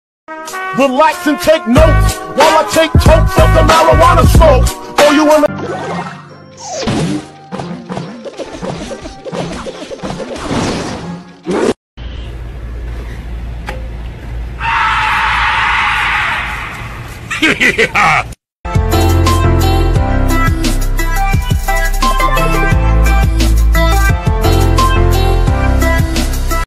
CLASH ROYALE IRL 😍#1 | @ClashRoyale | real life clash royale cards FT. mini pekka, skarmy
clash royale cards in real life
by